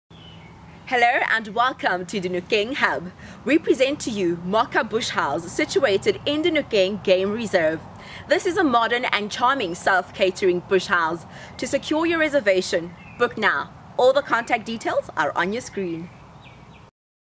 Mokha Bush House - Dinokeng Hub - Gauteng
MOKHA BUSH HOUSE

A nice modern and charming self-catering bush house in the Dinokeng Game Reserve. The house has 5 bedrooms and a lovely large open plan lounge, dining room and modern kitchen. There is also a large lapa and brand new swimming pool with a built in braai area.
A self-catering bush house (sleeps 10) that has 5 bedrooms and two bathrooms with a large open plan lounge, dining room and fully equipped kitchen. The kitchen is fully equipped with a stove, oven and a microwave. There is a double sleeper couch in the lounge
The bush house is situated on 21 hectares of dense bushveld and has lovely gardens and lawns and a brand new pool. DSTV is available. The house is not serviced on weekends. Over the weekend the house is available for a minimum of 2 nights only. R3000 per day.
Midweek Rate (Tuesday - Thursday) R2000 Per Night Minimum 2 nights stay
Mokha Guest house has two additional chalets that sleep 2 persons per chalet. These are available at R300 per person per day.

Mokha Guest House has a lapa and a braai area plus a lovely pool for those very hot days.

There is Eskom power at the camp and the kitchen also has a fridge and a deep freeze.

Bedding and towels are supplied, but please bring your own swimming towels.

Also please bring your own extra blankets during the winter season.

The camp is serviced during the week but not on the weekends.

Please note that this is a conservation area, do not take any wood from the bush, wood and charcoal is available to purchase at the local shopping centre.

Please keep the area pristine. Please co-operate in the spirit of conservation take only photographs and leave only footprints.
VERY IMPORTANT:
Please note this is a big 5 game reserve and dangerous game including lions are free roaming.

Do not leave the camp unless you are in a closed vehicle.

Do not walk outside of the gates.

Do not drive anywhere on the reserve except on the public roads or on the self-drive route if you have a permit.

Please contact Tamboti Bush Lodge for game drives.

MOKHA BUSH HOUSE
Price
Weekend Rate
R3000 Per Night (entire house)

Midweek Rate (Tuesday - Thursday)
R2000 Per Night (entire house)

Minimum 2 nights stay

Additional 2 chalets (sleeps 2 each)
R300 Per Person Per Night

Show less